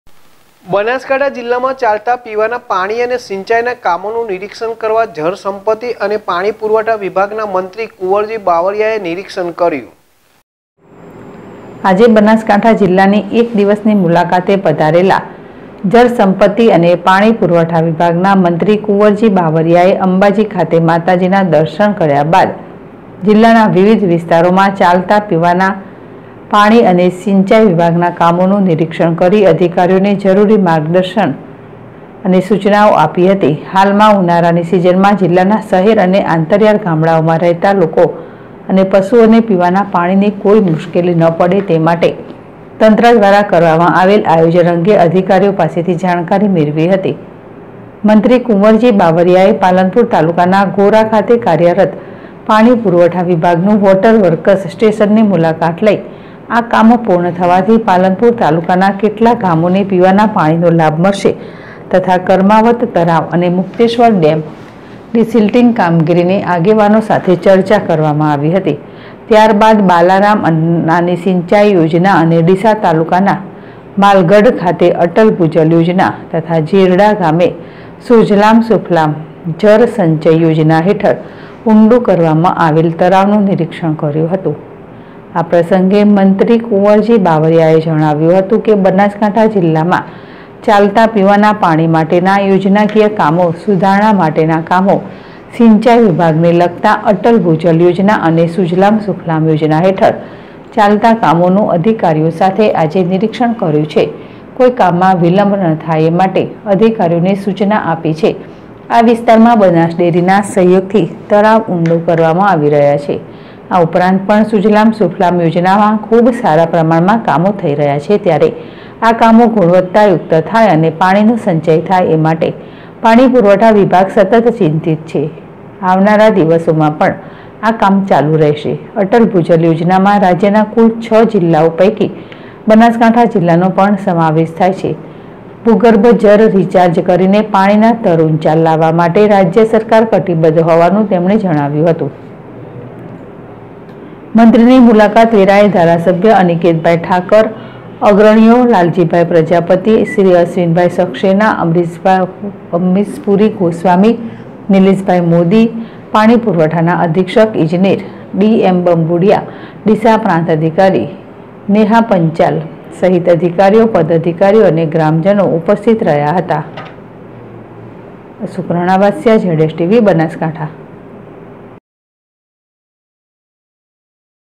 બનાસકાંઠા જિલ્લામાં ચાલાતા પીવાના પાણી અને સિંચાઇના કામોનું નિરીક્ષણ કરતા જળ સંપત્તિ અને પાણી
બનાસકાંઠા જિલ્લામાં ચાલાતા પીવાના પાણી અને સિંચાઇના કામોનું નિરીક્ષણ કરતા જળ સંપત્તિ અને પાણી પુરવઠા વિભાગના મંત્રીશ્રી કુંવરજી બાવળીયા